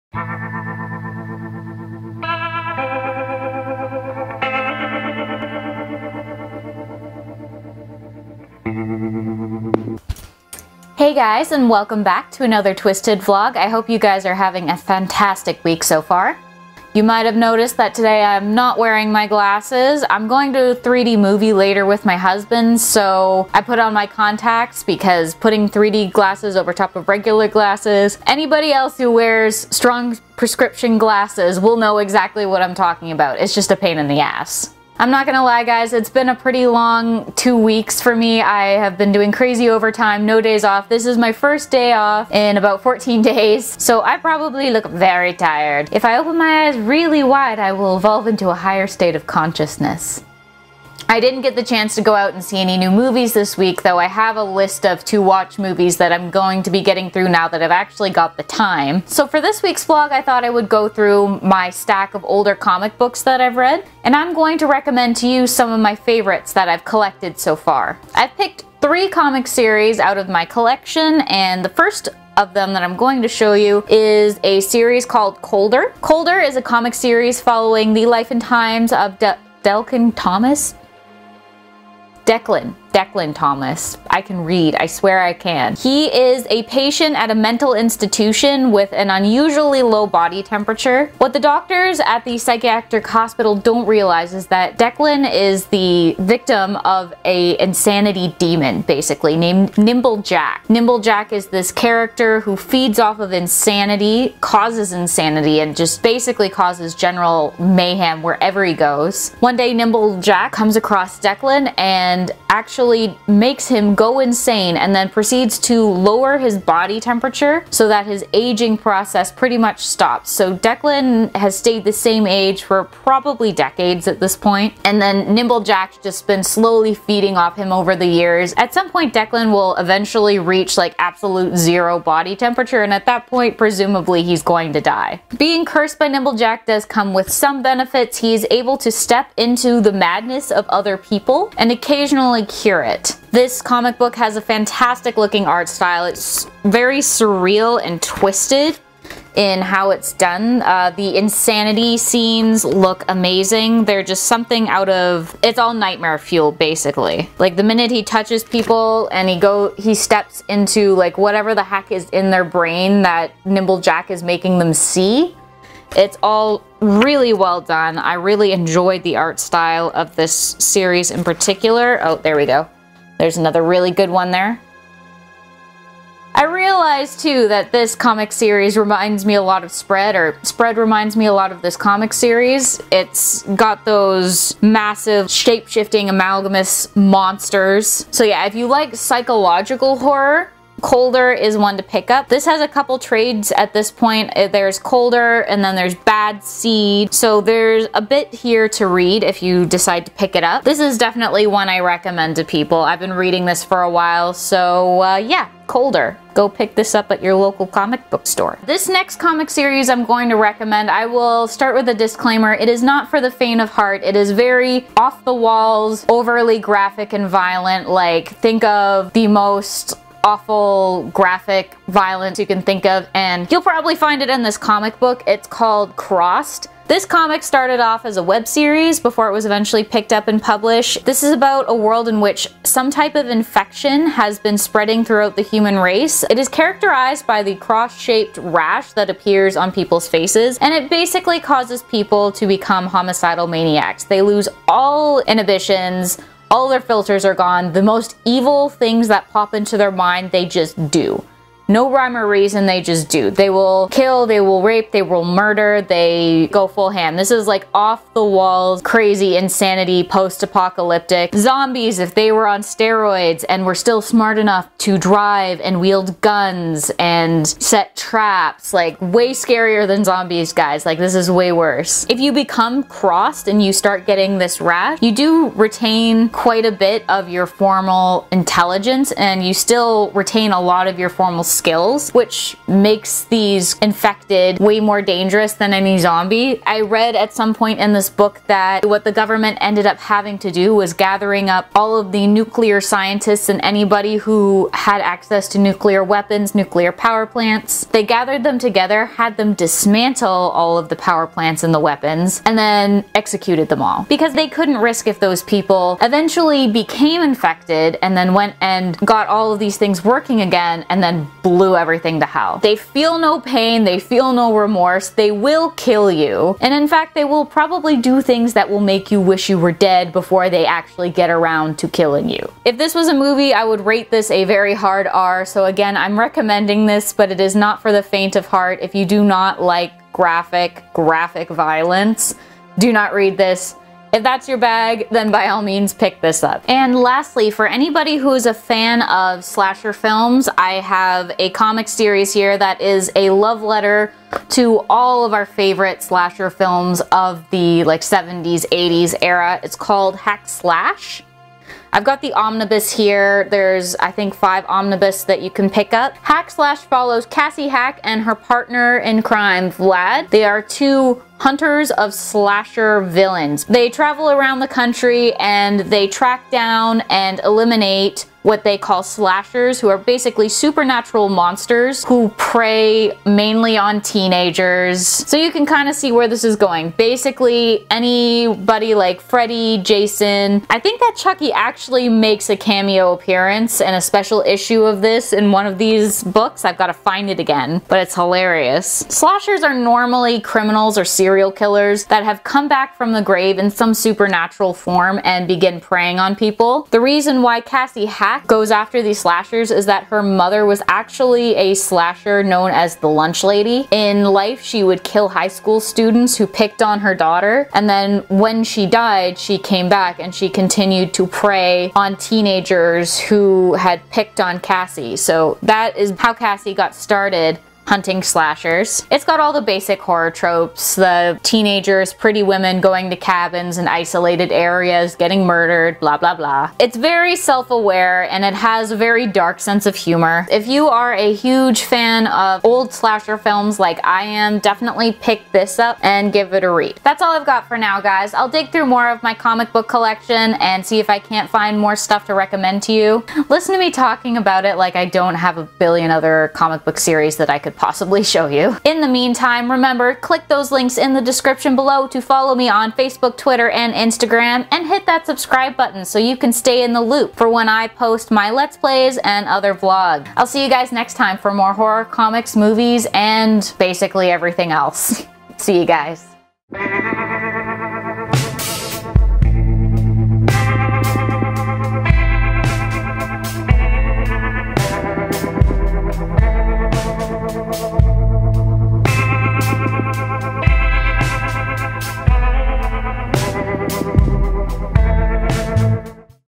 Colder, Crossed and Hack/Slash Comic | Vlog
This week I went through my comic stash and brought out my copies of Colder, Crossed and Hack/Slash. These are some of my favourite horror comics!